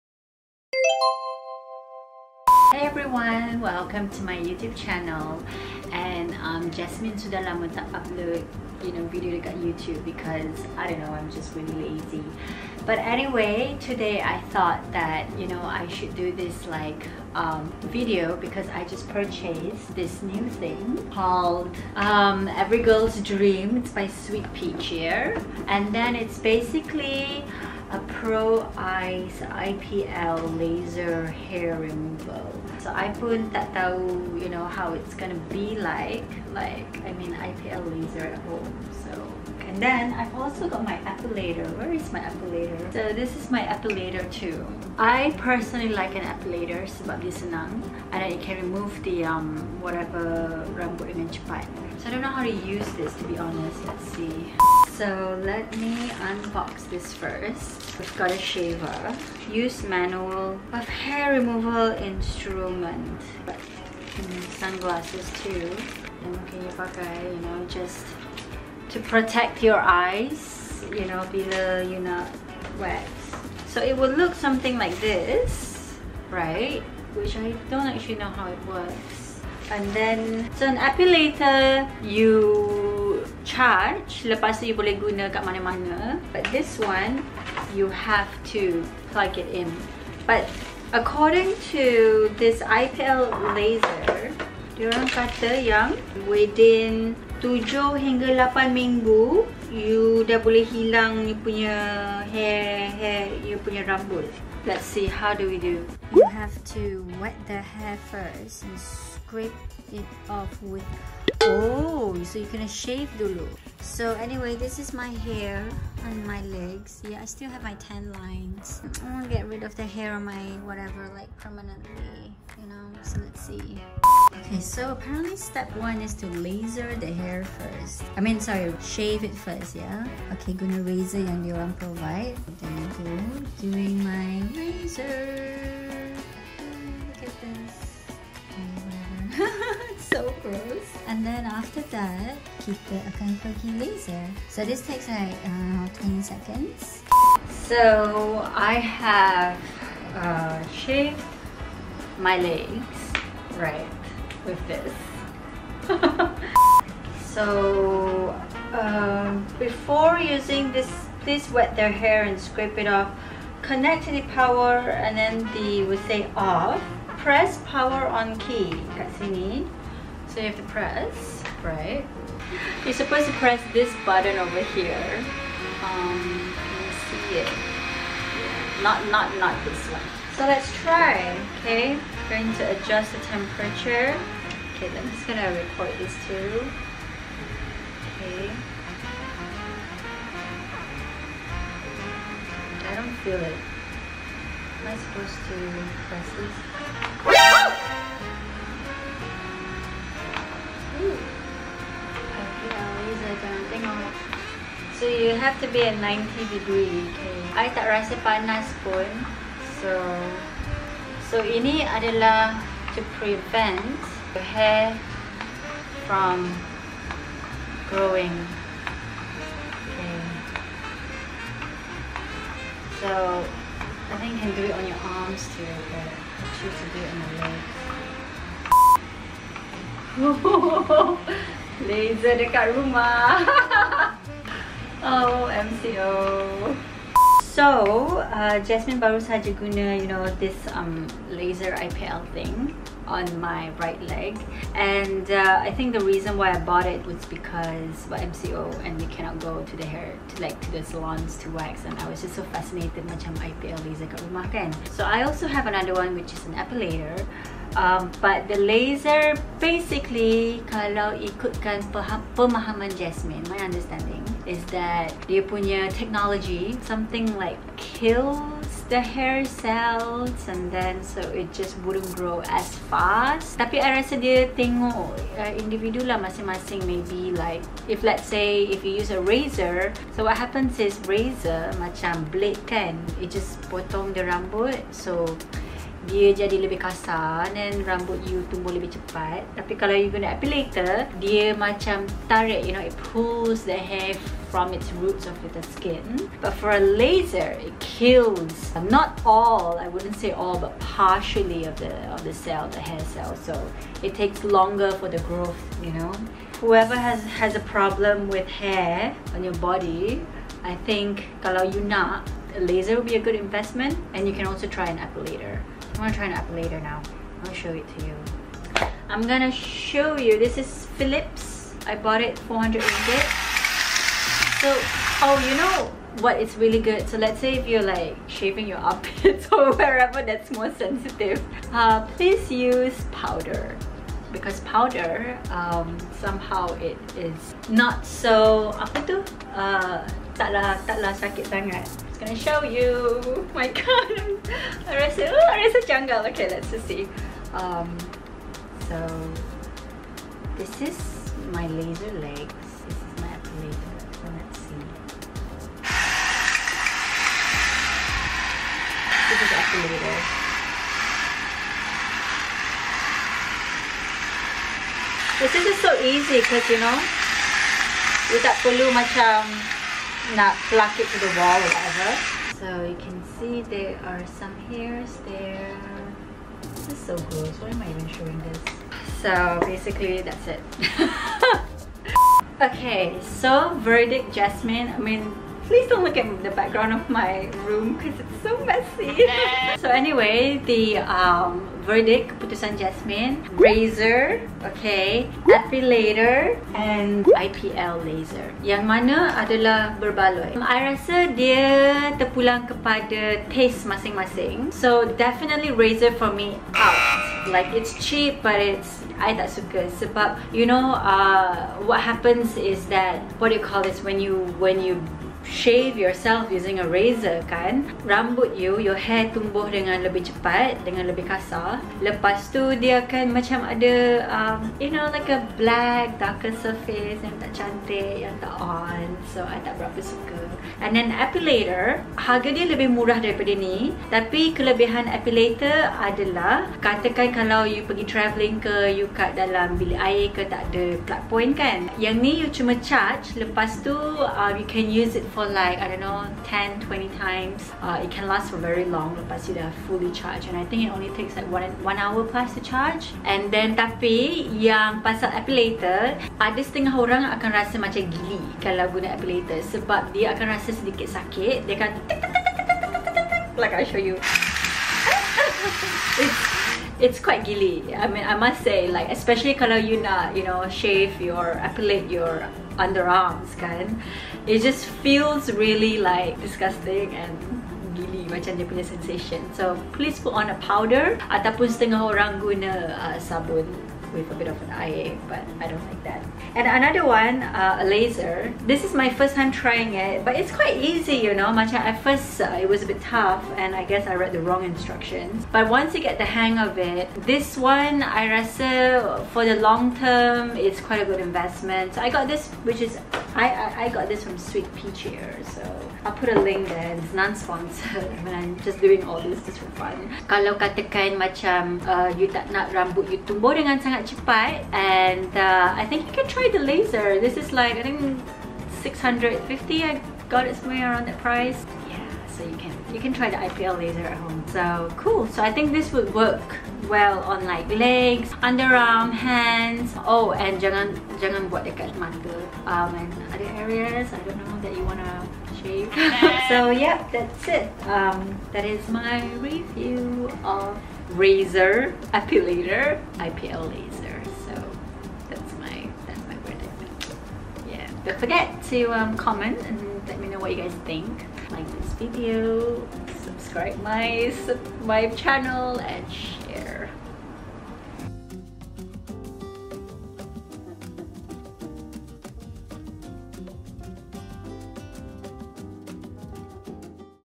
Laser Hair Removal At Home | DOES IT WORK??
Hi semua,

Anyone here pernah cuba buat laser hair removal dekat rumah? Jasmine beli satu from Sweet Peachier untuk cuba and compare with my Philips epilator, kalau korang nak tahu jadi ke tak, kena tengok sampai habis!

Hope korang enjoy this video. Stay safe and take care :)